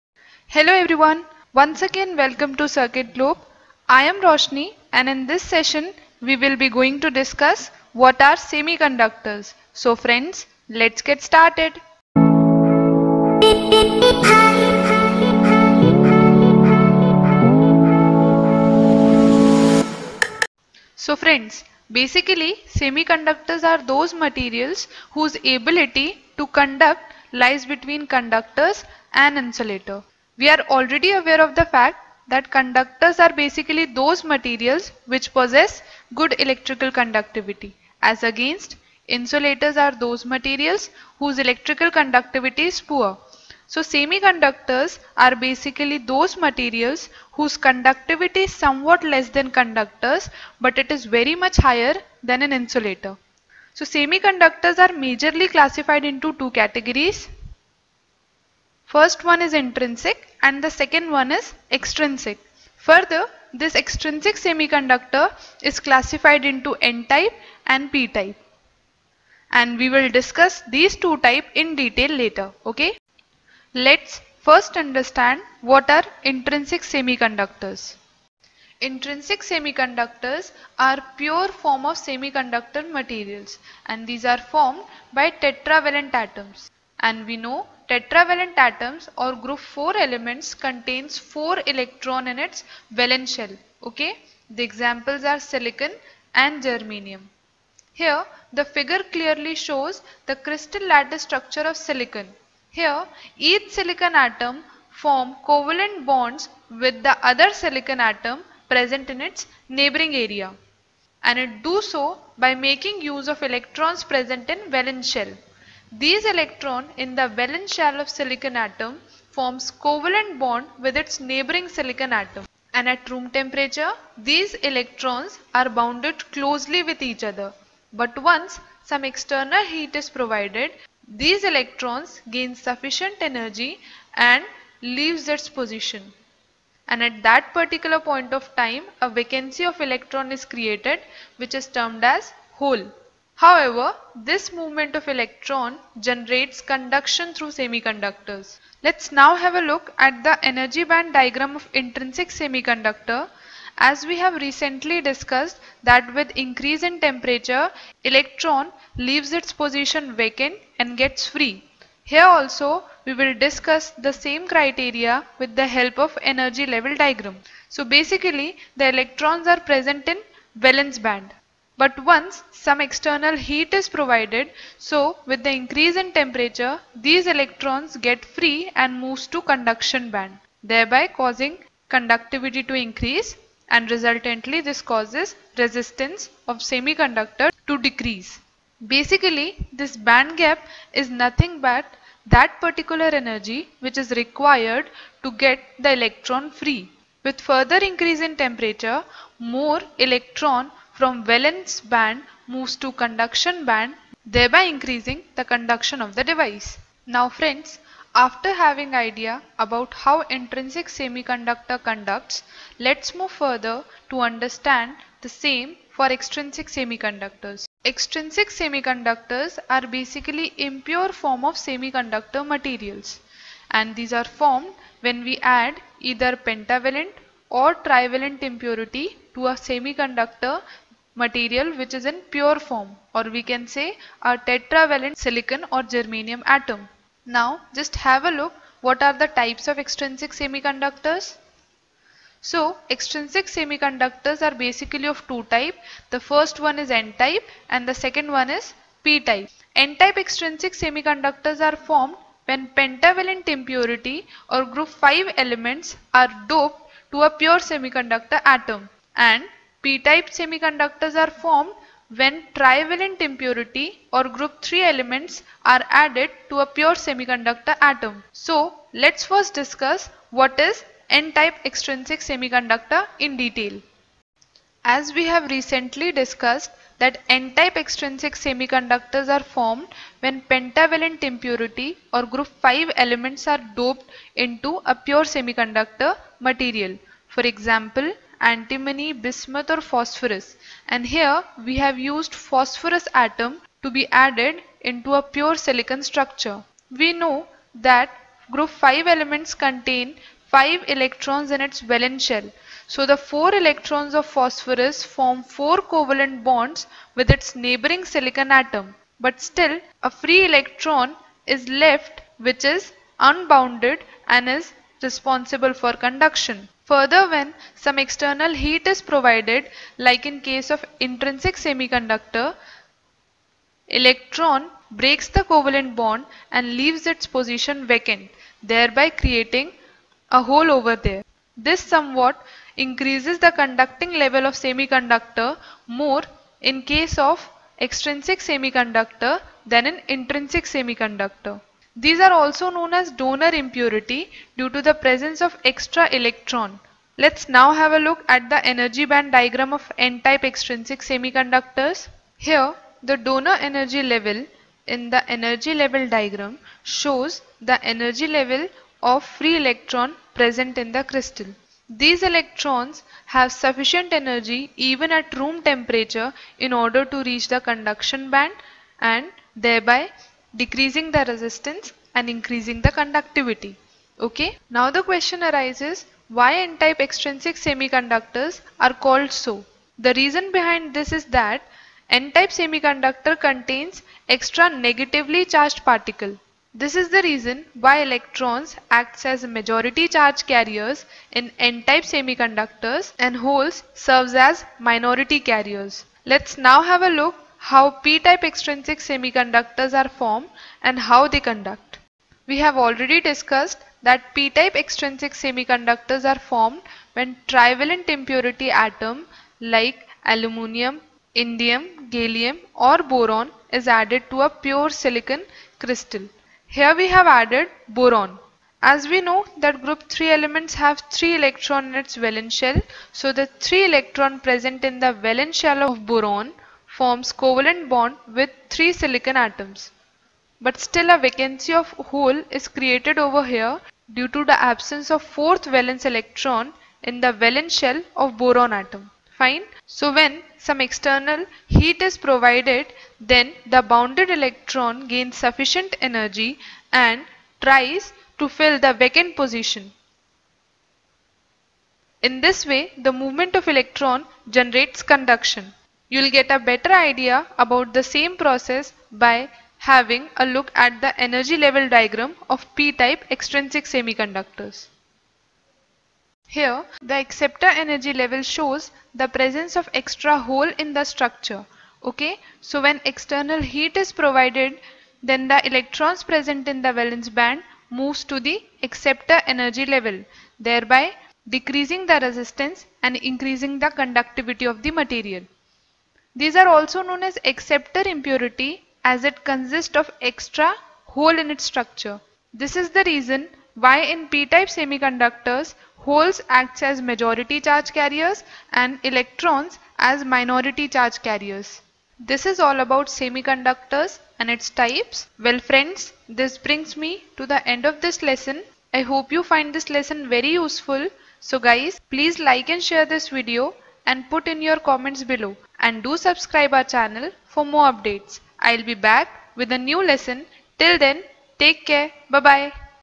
What are Semiconductors? Intrinsic and Extrinsic Semiconductors.
This video is all about Semiconductors where you will get to know about the meaning and types of semiconductors i.e., intrinsic and extrinsic semiconductors. Further in this session, you will find the types of extrinsic semicondcutors i.e., n- type and p-type semiconductors.